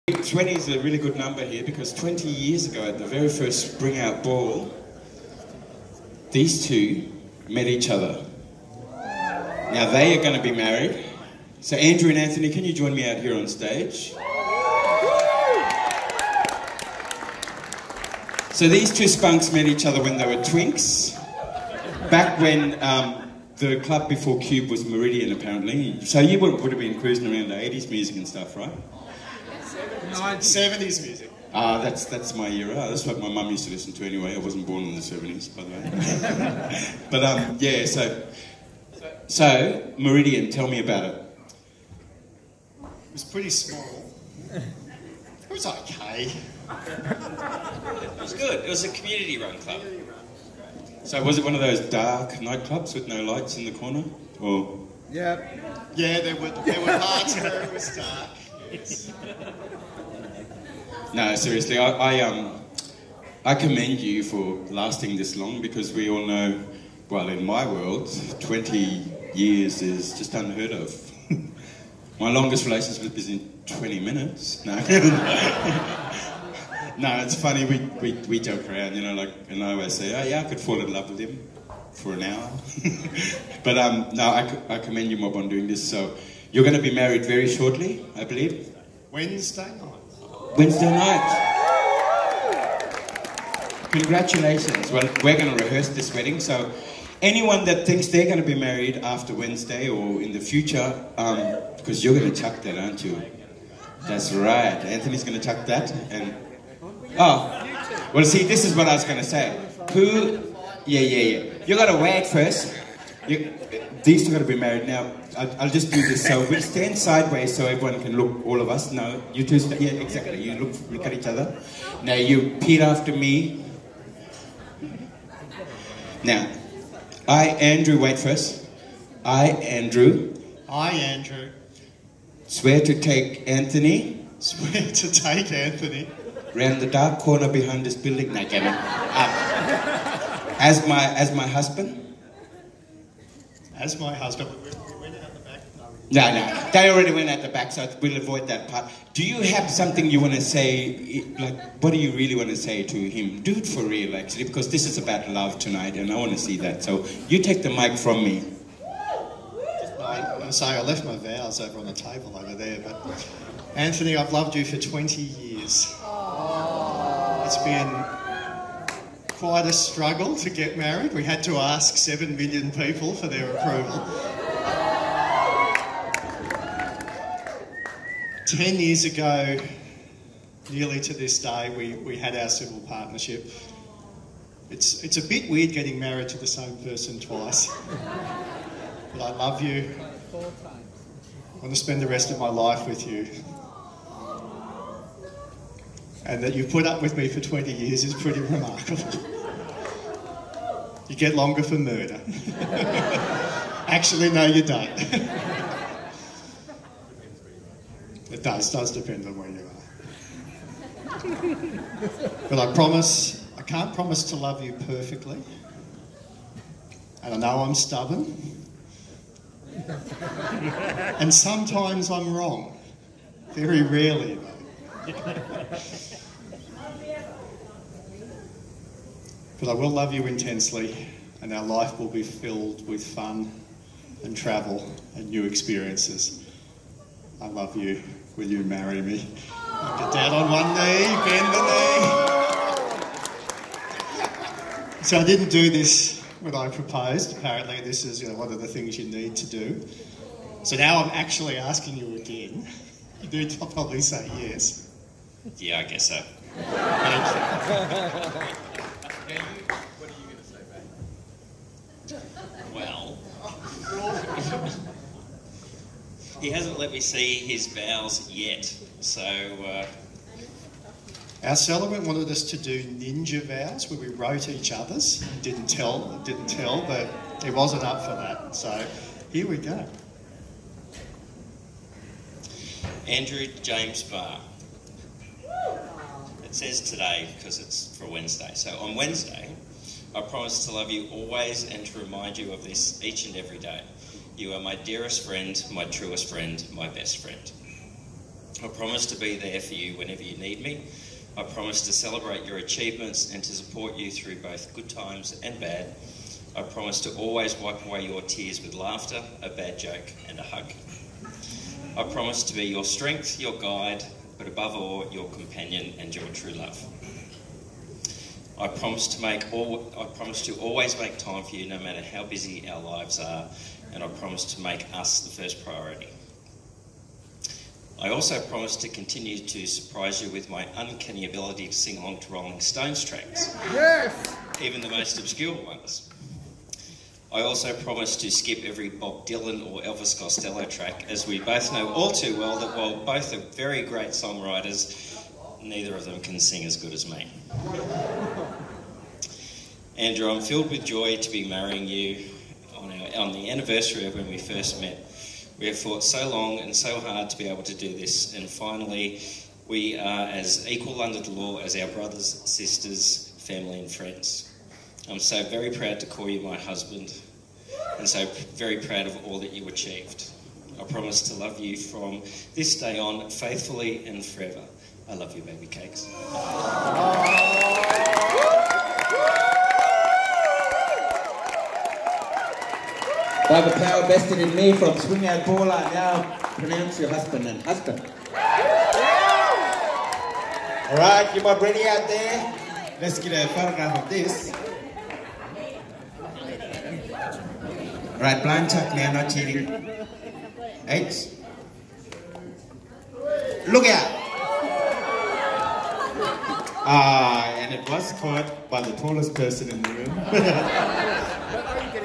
Andrew Barr & Anthony Mock Wedding 2019 SpringOUT Ball [exclusive footage]
Chief Minister of the ACT Andrew Barr and his partner Anthony performing in a mock wedding [prior to their actual wedding] at the SpringOUT Ball 2019.